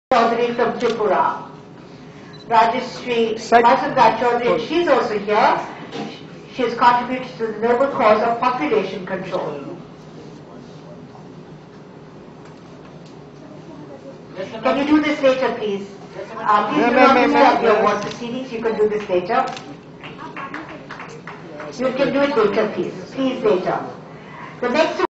CHIKITSAK RATAN AWARD CEREMONY :18TH JANUARY;2010
LOCATION ; REGENCY HALL;
KONNAUGHT HOTEL;
KONNAUGHT PLACE;
NEW DELHI.
In this VDO :Dr.Rajasree majumder was conferred CHIKITSAK RATAN AWARD 2010( for her PERSONAL ACHIVEMENT IN THE FIELD OF GYNAECOLOGY 7 POPULATION CONTROLL ) byDr.G.V.G.Krishnamurthy:His excellency Mr.S.A.Beg (AMBASSADOR : KAZAKSTAN) and other diplomats.